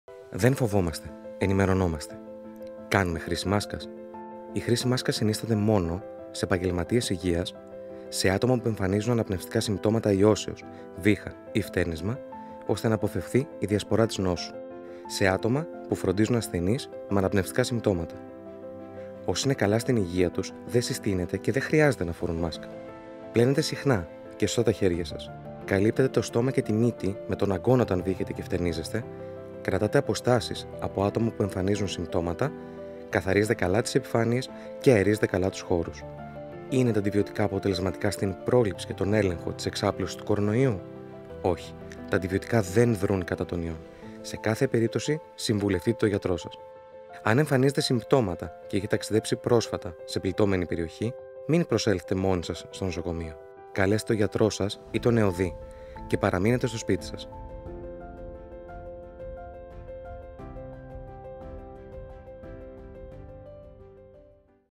Κορονοϊός - Δεν φοβόμαστε: ενημερωνόμαστε.
Τι κάνουμε για πρόληψη;
Πότε κάνουμε χρήση μάσκας;
Είναι τα αντιβιοτικά αποτελεσματικά;
Αν εμφανίζετε συμπτώματα και έχετε ταξιδέψει πρόσφατα σε πληττόμενη περιοχή, μην προσέλθετε μόνοι σας στο νοσοκομείο: Καλέστε το γιατρό σας ή τον ΕΟΔΥ και παραμείνετε στο σπίτι σας.